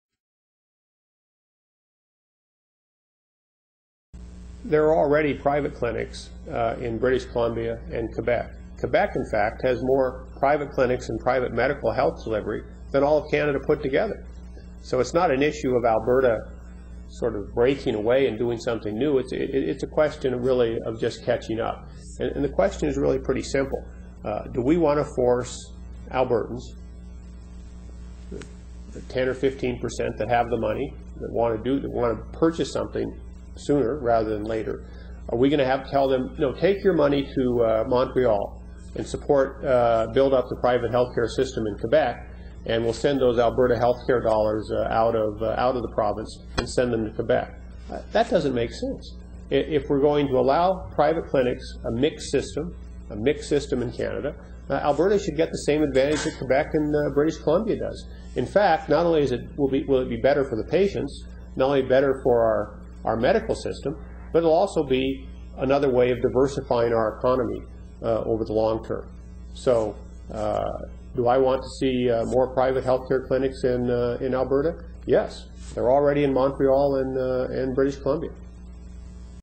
Ted Morton talks about private clinics
Ted on why we shouldn't be scared of "private clinics" in Alberta.